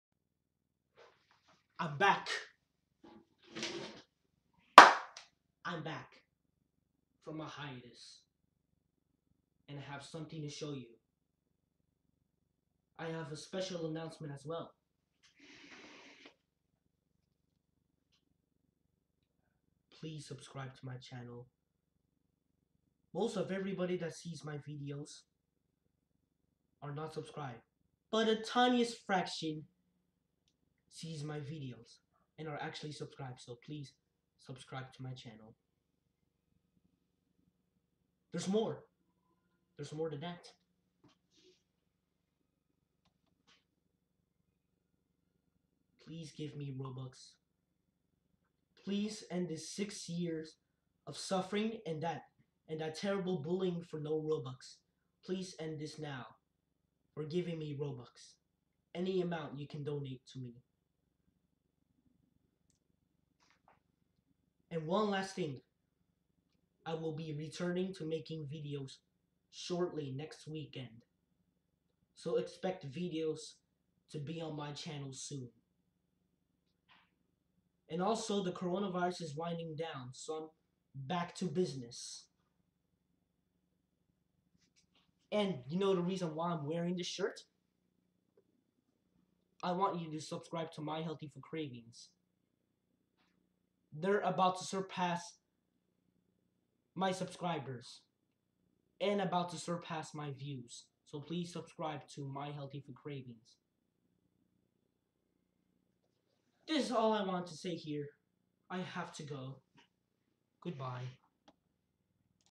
I'm Back! (Special Announcement also!)
i'm finally back from my hiatus i will be returning to making videos shortly

Please Give me Robux!